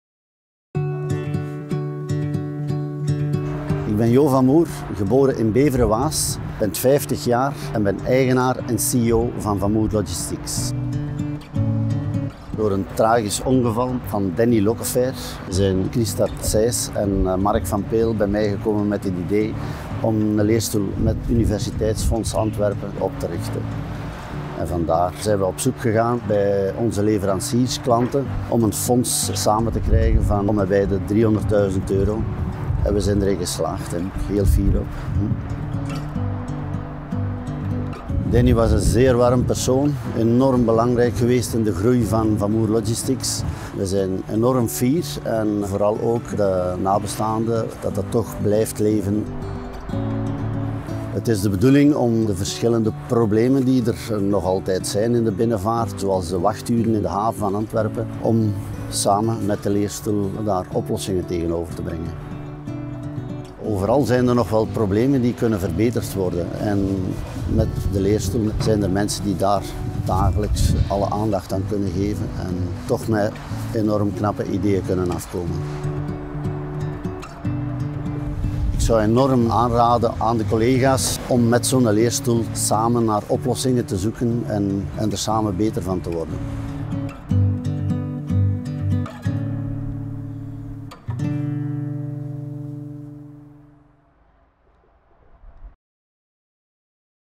Today, let’s care about tomorrow - testimonial Jo Van Moer
Jo Van Moer, CEO of Van Moer Logistics explains why, as an entrepreneur, he chooses to collaborate with the University Fund Antwerp. After all, doing business is also daring to invest in tomorrow! Discover what the University Fund can do for you